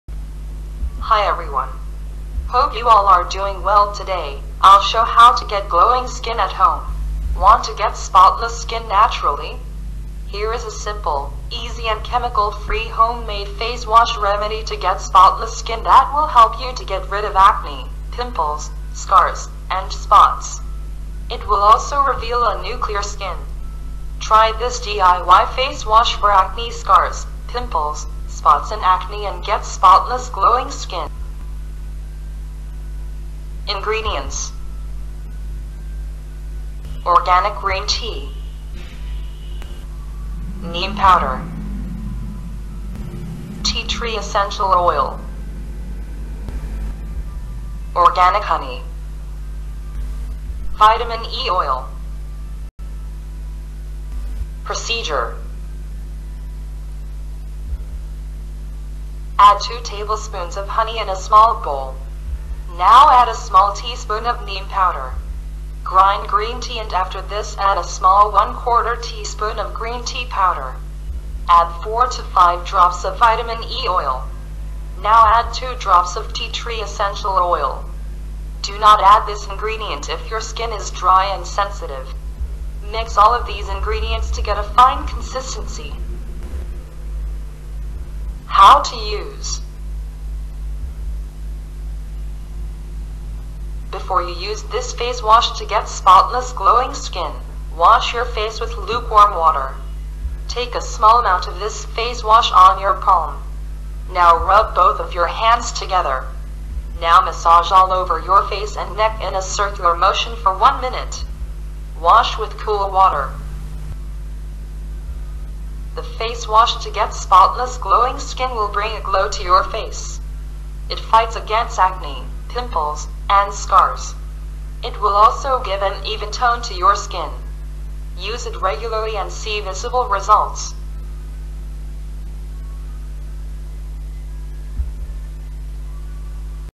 How to Get Glowing Skin at Home Naturally within 3 Days
Hello everyone, In this video I will show How to Get Glowing Skin at Home within 3 days. Every one want to get glowing skin but some women and women take care properly for this. Most of the women fell concern for glowing skin and glowing face. I hope this video will help you so much to get Glowing Skin naturally at Home.

Want to get spotless skin naturally? Here is a simple, easy and chemical-free homemade face wash remedy to get spotless skin that will help you to get rid of acne, pimples, scars, and spots.     It will also reveal a new clear skin.     Try this DIY face wash for acne scars, pimples, spots and acne and get spotless glowing skin.

Ingredients:

Organic Green Tea
Neem Powder
Tea Tree Essential Oil
Organic Honey